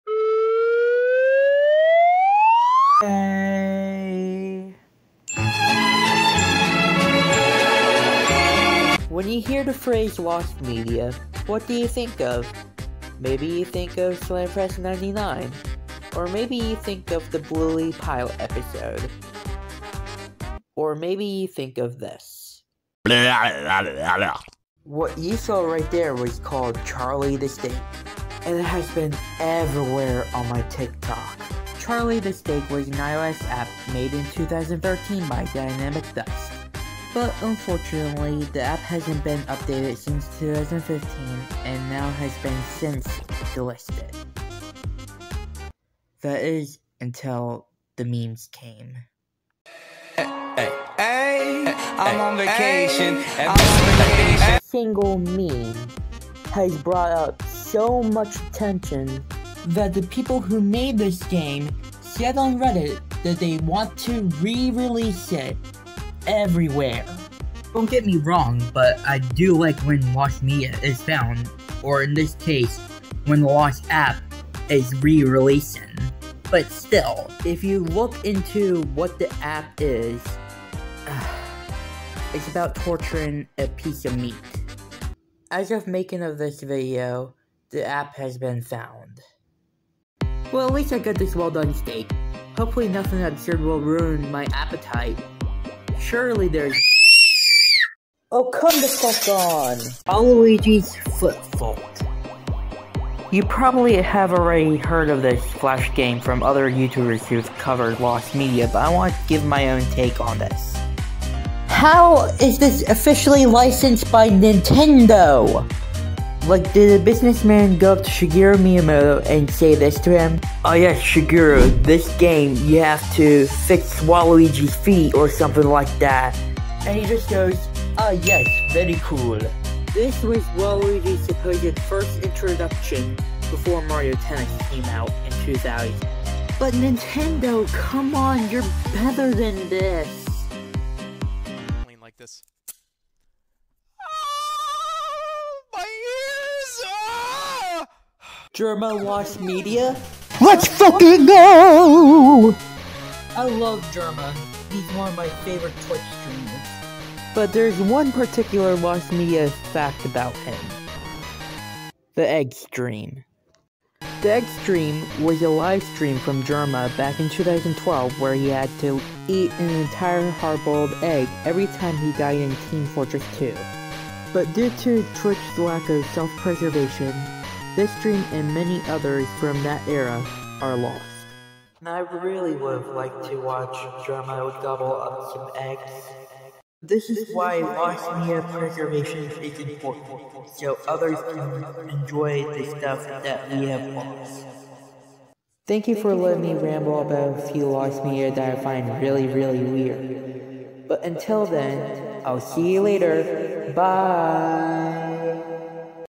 Lost Media I Find Weird As Hell
When it comes to lost media, there are some very odd ones. So I would like to give my own thoughts on what I think of these three lost medias.